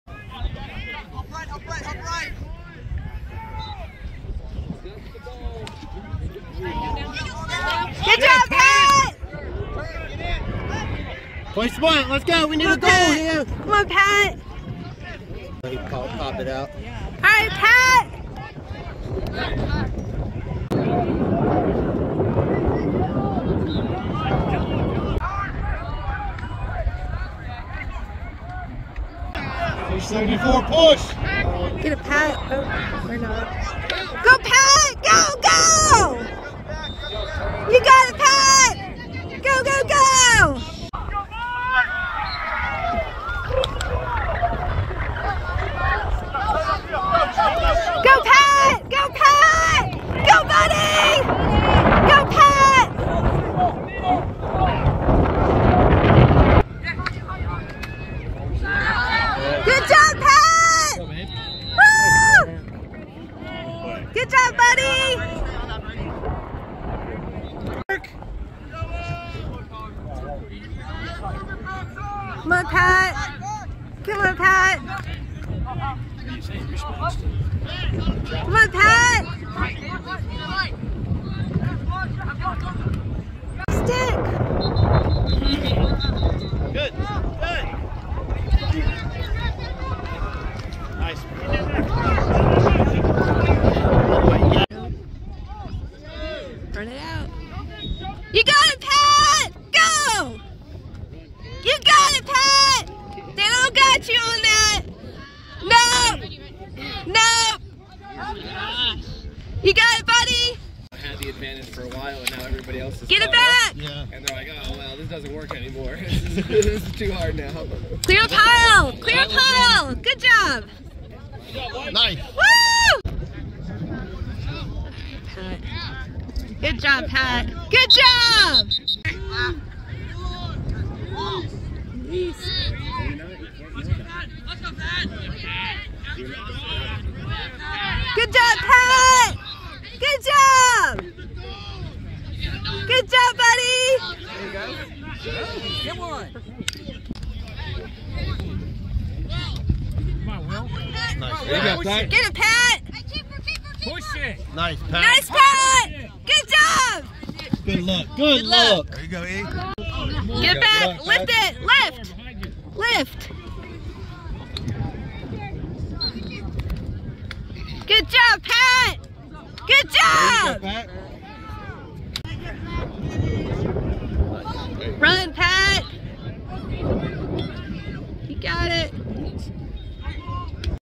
Highlights from Day 2 and 3 from Club World Championship (Dec 22)
Patrick Flatley highlights from Club World Championships (Austin, TX) Dec 22

Team Eighteen